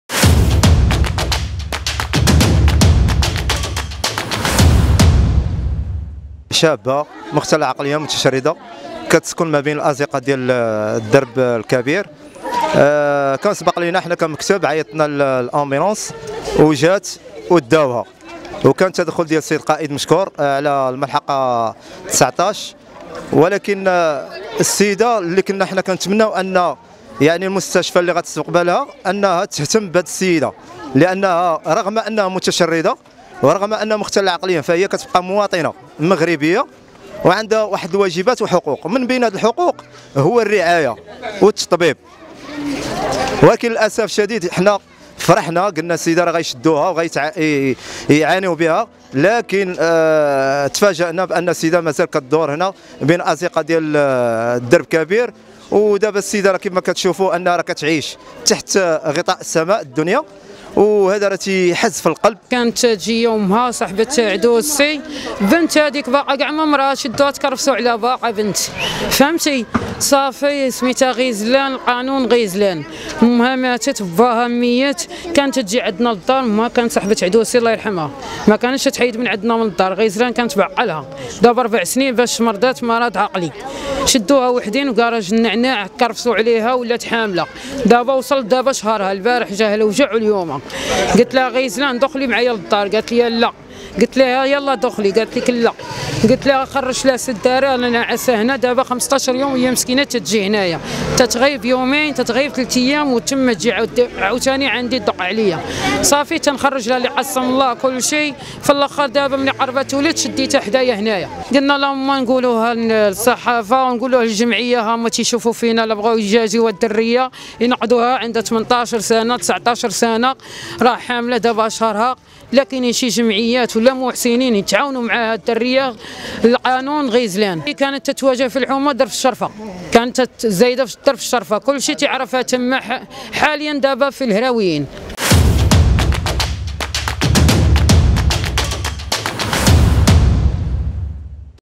شابة مختلة عقليا تتخلى عنها وزارة الصحة تنام في العراء بدرب الكبير
أخبار المغرب
# أخبار عاجلة